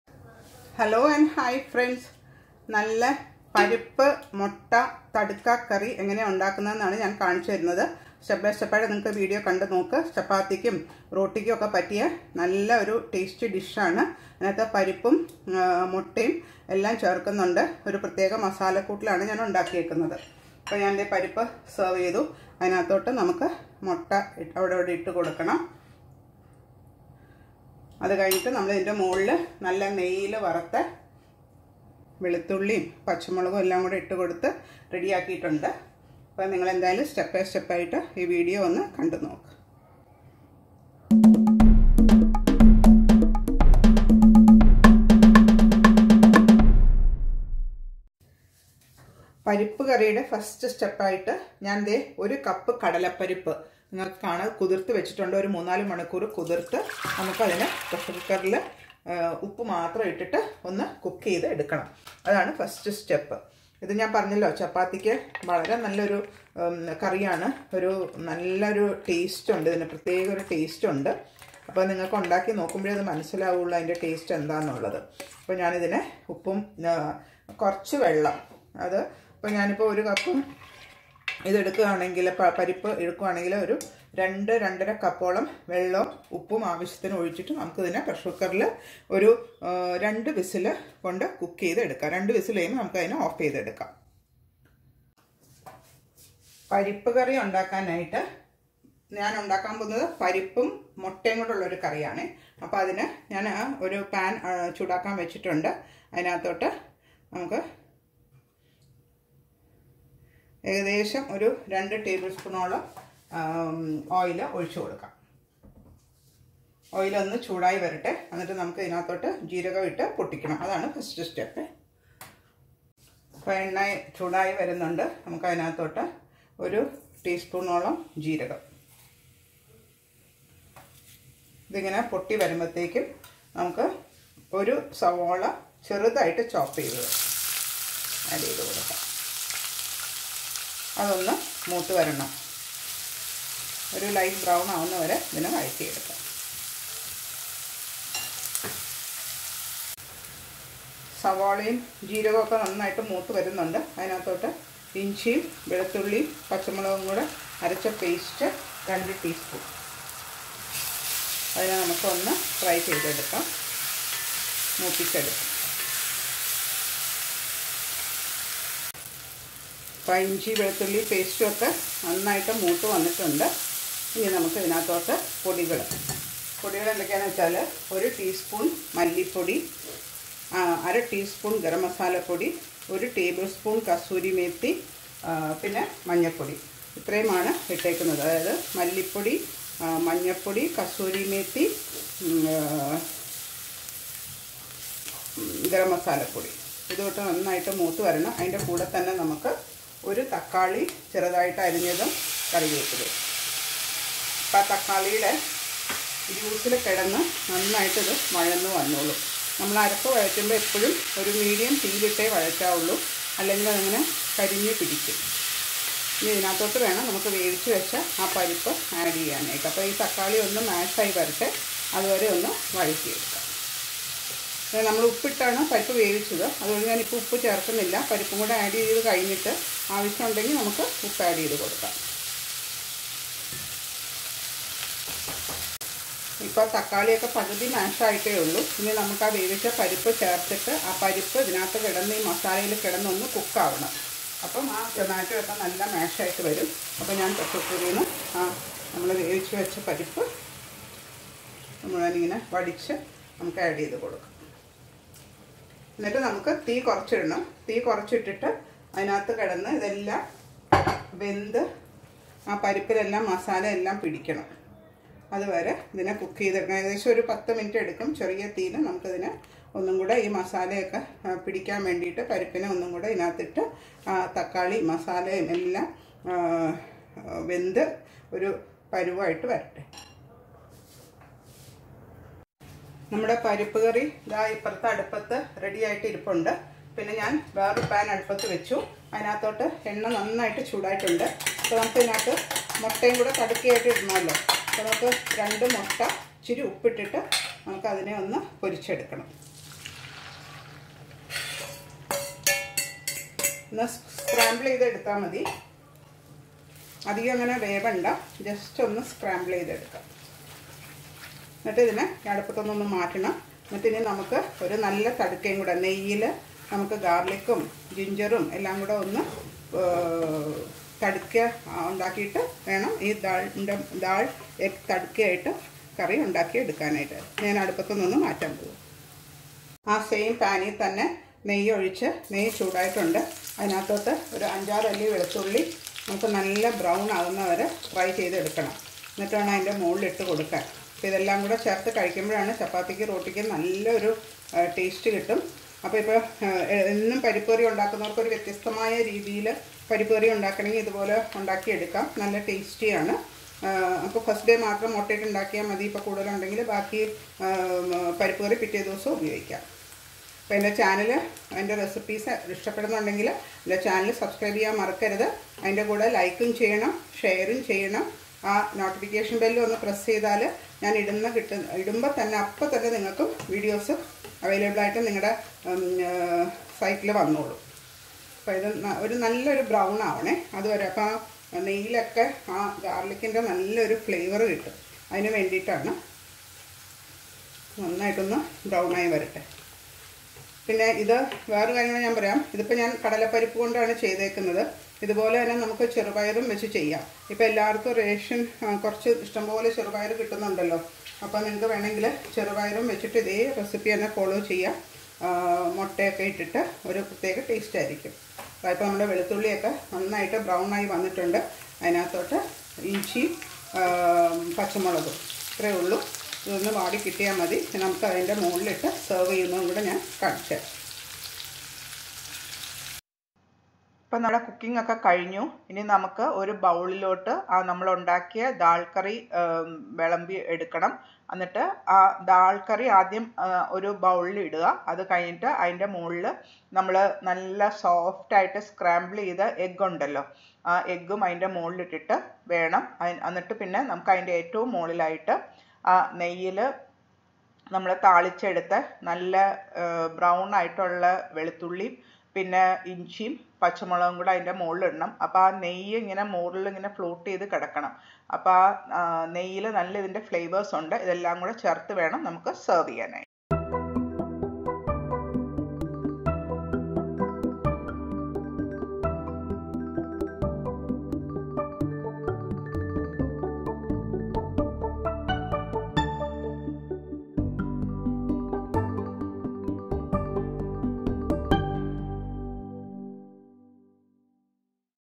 Egg Dal Tadka Recipe || How To Make Egg Dal Tadka
Egg Dal Tadka is a popular Indian dish at home  and made from dal and scrambled eggs. This flavorful and aromatic tadka is always go with roti or chapati. Make this easy recipe in your own kitchen and enjoy a great dinner with your family.
chana dal     1 glass soaked for 3 hours
salt
ginger garlic chilli paste    2 tsp
coriander powder    1 tsp
turmeric
curry leaves
For scrambled egg
egg   2
salt
ghee   1 tsp
For Tadka
sliced garlic    5
chopped ginger   1 tsp
sliced green chillies   3
Method
Cook chana dal with salt  & water in cooker for 3 to 4 whistles. Heat oil in a pan, temper cumin seeds. Add onion and saute till brown. Add ginger, garlic chilli paste and saute. Add spice powders and saute. Add tomato and curry leaves. Saute well. Add cooked chana dal and simmer for 5 to 8 minutes. Remove dal from fire. Heat 1 tsp ghee in another pan. Beat eggs with salt and ad to pan. Cook for 3 minutes and remove from fire. Heat ghee in pan, fry garlic till brown. Add ginger chillies and remove from fire. In a serving bowl ,pour dal curry. Garnish dal with scrambled eggs and tadka.
Serve hot with roti or chapati.